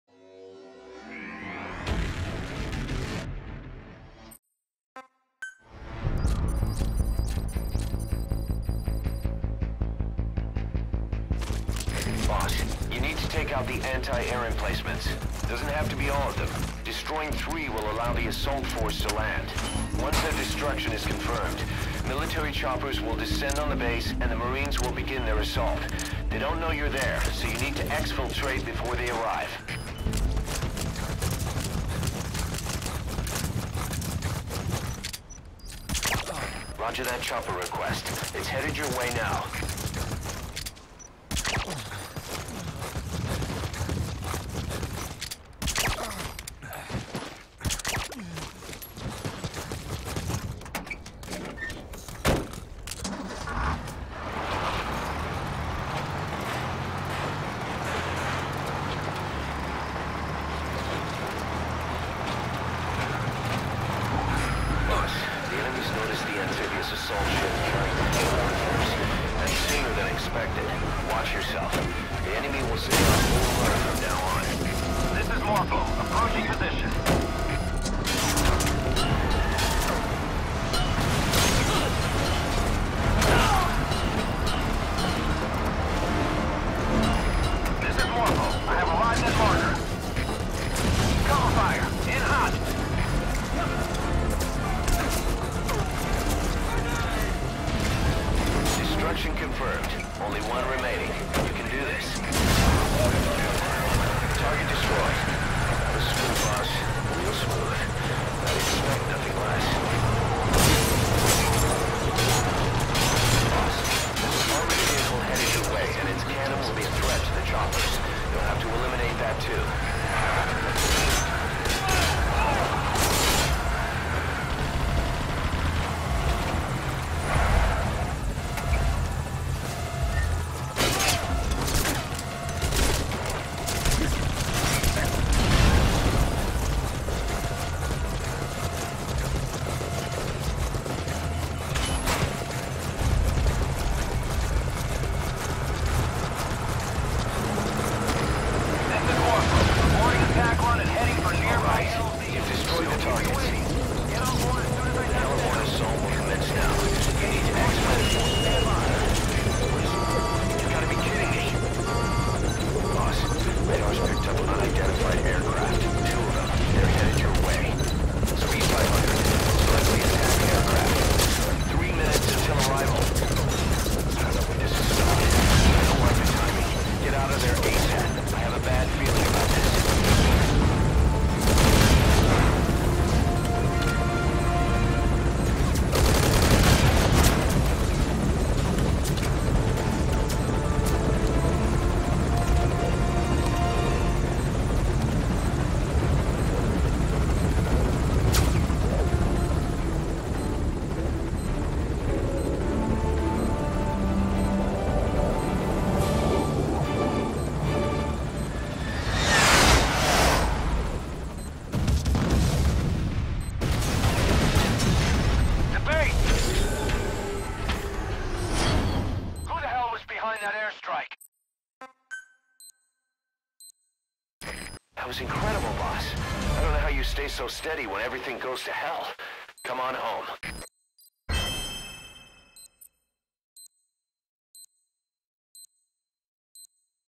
"That was smooth boss, real smooth"
"I'd expect nothing less"

Evidently I recorded this with the wrong settings, I'm using OBS and I forgot I had the quality down for streaming.  I'll probably rerecord this at some point.

Few slip-ups, accidentally mis-selected some items at points, hit the ramp with that first truck (you can drive it just right where you essentially drift into the first anti-air cannon), and a multitude of problems with the second truck (guy jumped in the way ruining the no-kill run, and sometimes the truck won't explode properly, meaning I had to go back and chuck a grenade) but otherwise this run went well, one of the better ones I've had.